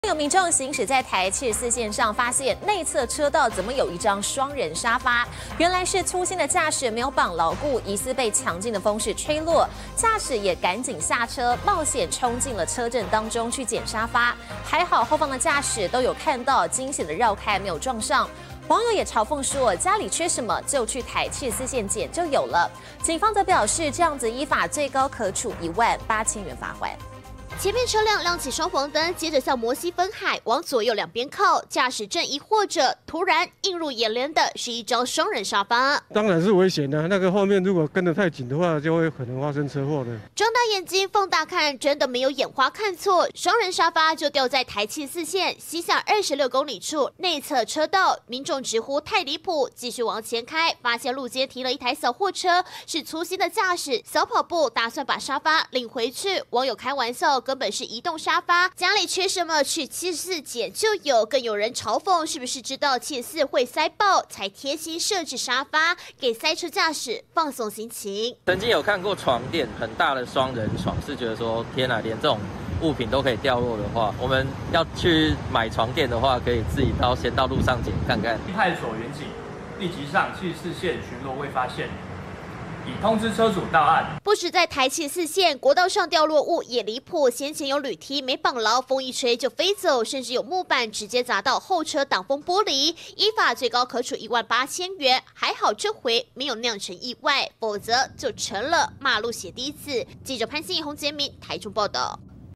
嚇!台74線驚見雙人沙發掉路中 駕駛急領回
太離譜！台中有民眾行駛在台７４線上，發現內側車道有一張雙人沙發，原來是粗心的駕駛沒有綁牢固，疑似被強勁風勢吹落，他趕緊下車，冒險衝進車陣中撿沙發，還好後方駕駛都有看到，驚險繞開沒有撞上。網友就嘲諷，家裡缺什麼，去台７４線撿就有，警方表示依法最高可處一萬八千元罰款！